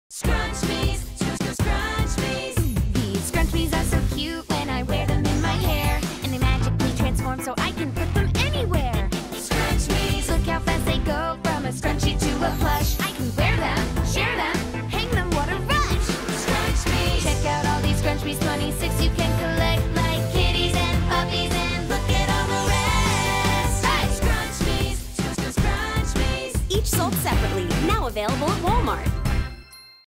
Scrunchmiez
We relived our 90s scrunchy fantasies for this new brand from Moose Toys. Let's just say, we weren't too upset about doing our usual product testing. And neither were our 2 talent, who came to the office with pep in their steps for a combo wardrobe fitting and hair test. With limited time on set, we elected to pre-plan the ideal styles and dos to best feature the product in each scene. Our motto is always: whatever can be approved in advance should be tried and tested and leave nothing to chance on the day!

Director/Producer: Zac Reaney-Barker
DOP: Martin Wojtunik
Client: Moose Toys
Creative: WhirlyGig Creative
Video Type: Commercial
Shoot Location: Pie in the Sky Studios